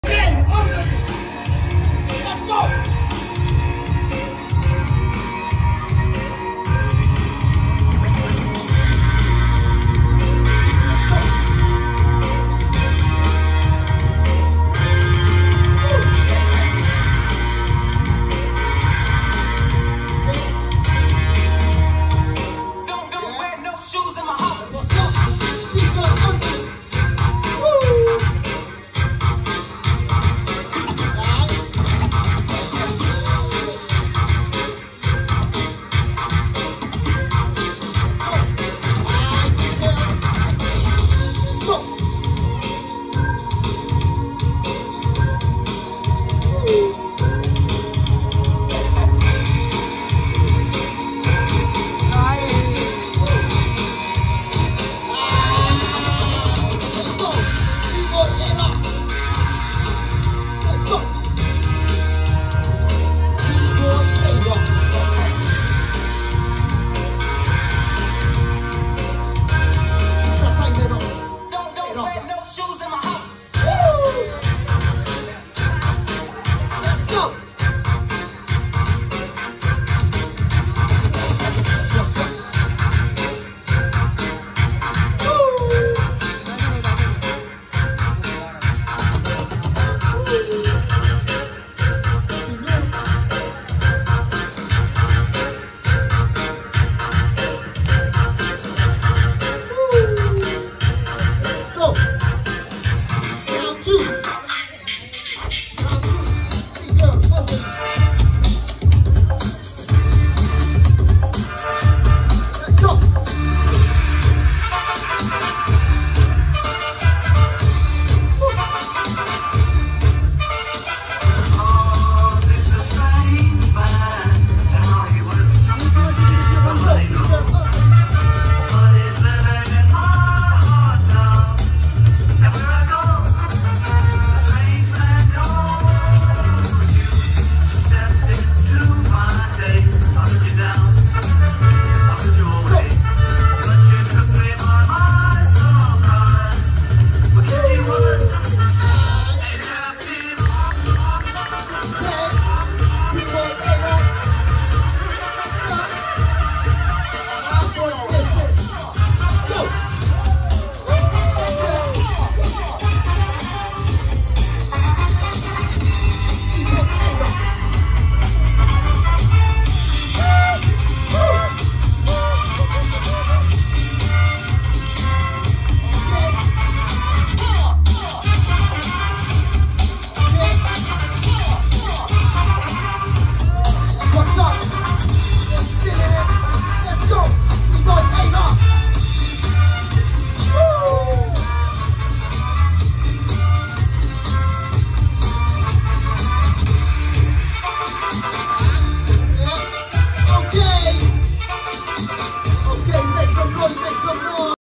ANJALI vs A ROCK [TOP8] Nepal Battle Station Vol.6 / breakTV.
Astro Boyz “14th Anniversary Jam” NEPAL BATTLE STATION VOL.6 2024.

Organized by - Astro Boyz Crew (Nepal 🇳🇵)

Judge - BBoy Born (MZK x Rivers Crew)

Emcee- Scooby / Astro
DJ - Arun / Motion

Venue - Reggae Bar (Thamel Kathmandu)

-breakTV.